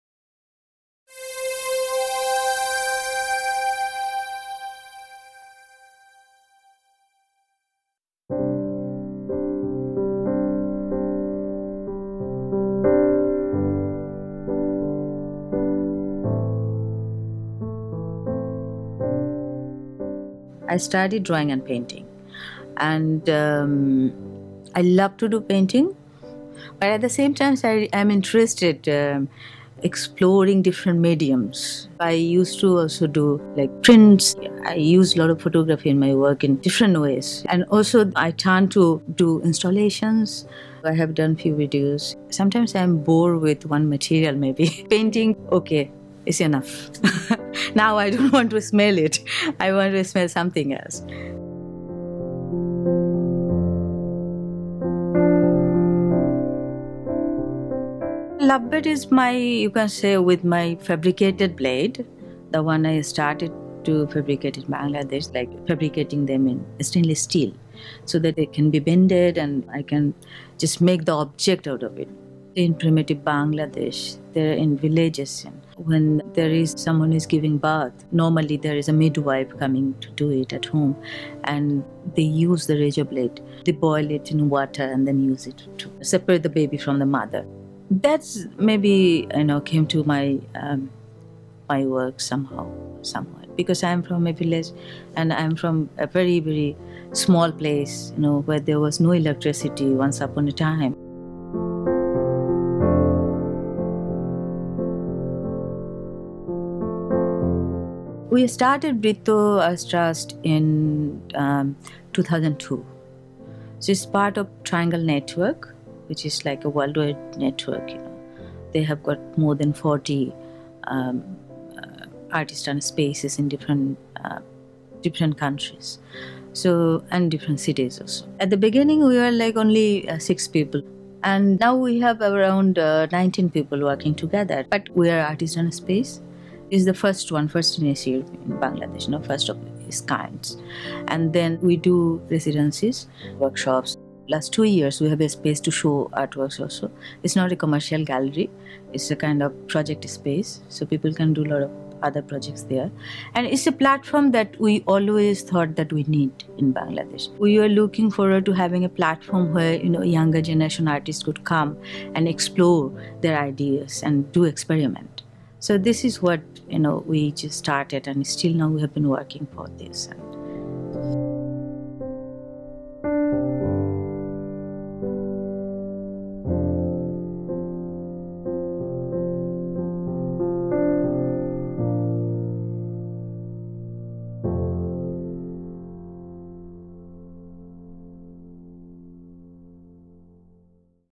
In Conversation with Tayeba Begum Lipi | No Country: Contemporary Art for South and Southeast Asia
Tayeba Begum Lipi deep dives into her art and her artwork, Love Bed, featured in No Country: Contemporary Art for South and Southeast Asia, the inaugural touring exhibition of the Guggenheim UBS MAP Global Art Initiative. No Country features works by 13 artists from Bangladesh, Cambodia, India, Indonesia, Malaysia, Myanmar, Pakistan, the Philippines, Singapore, Thailand, and Vietnam.